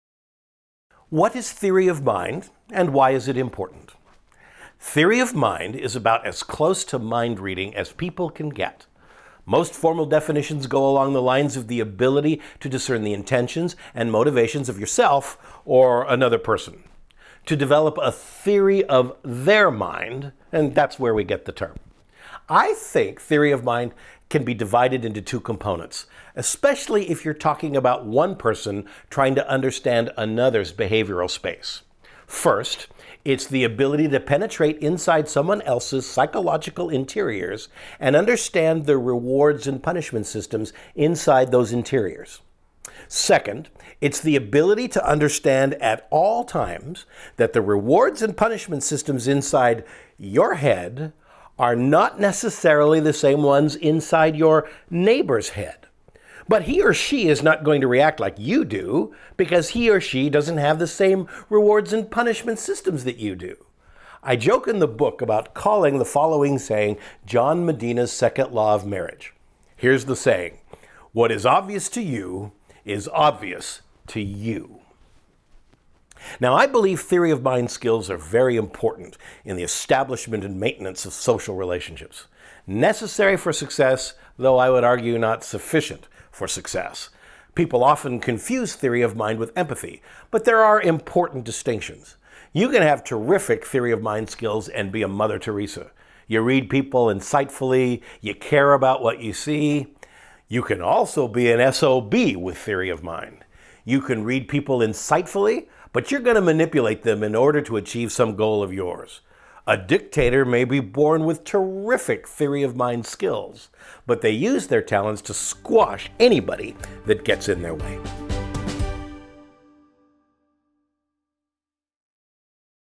Theory of Mind - John Medina, author of Brain Rules
John Medina, author of the NYT bestseller, Brain Rules, explains Theory of Mind.